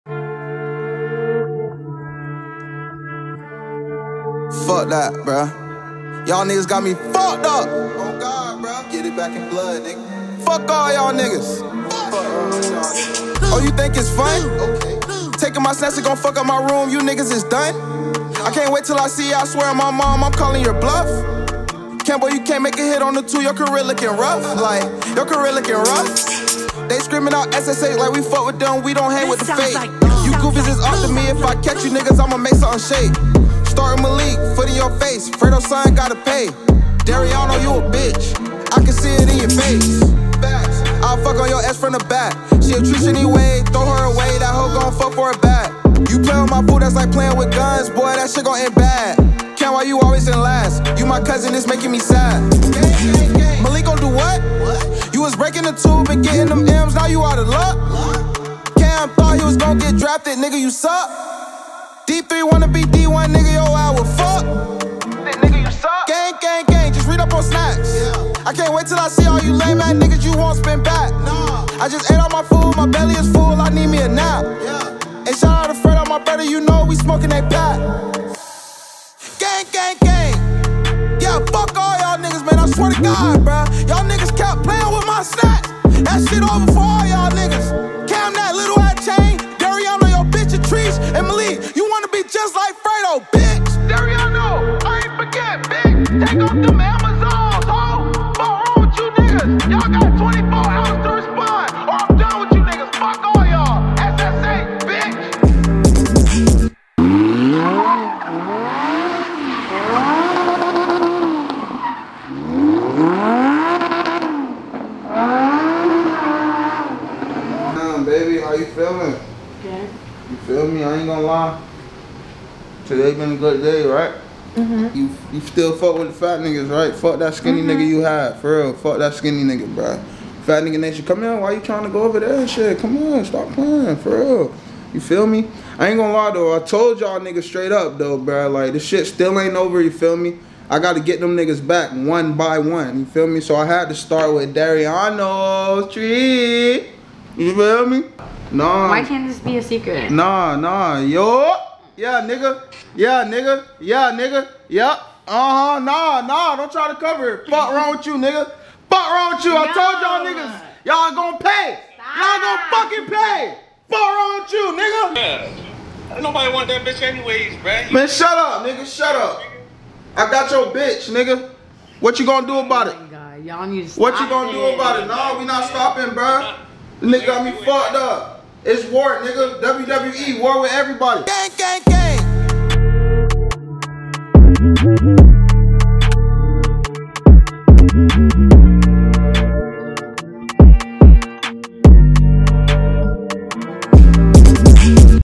HE DROPPED A DISS TRACK ON SSH 👀😳
THINGS ARE GETTING HEATED!!!
SSH Member Swift Quis Drops a diss track on other SSH members after getting his snacks destroyed.